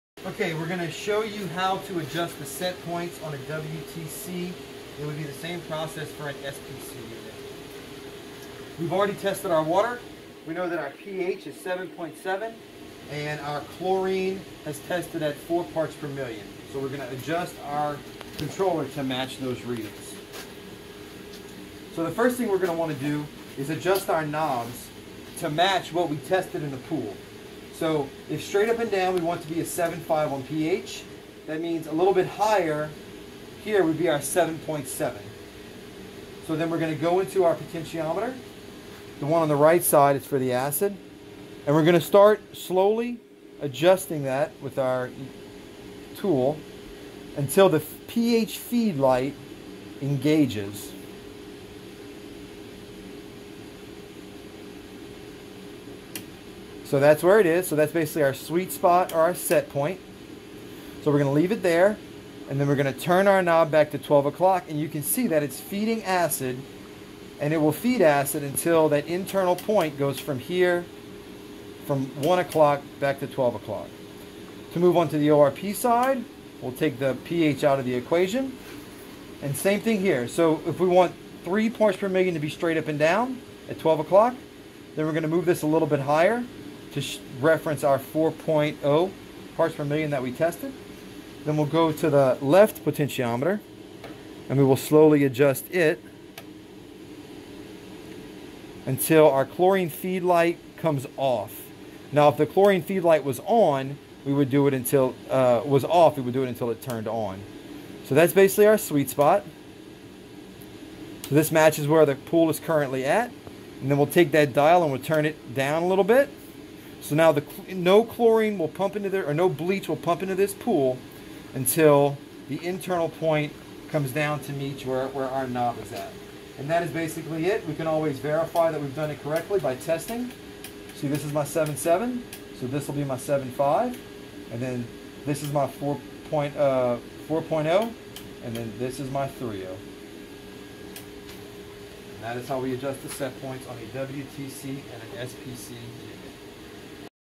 Adjusting Setpoints
Richard demonstrates setting mV setpoints w/o a signal generator.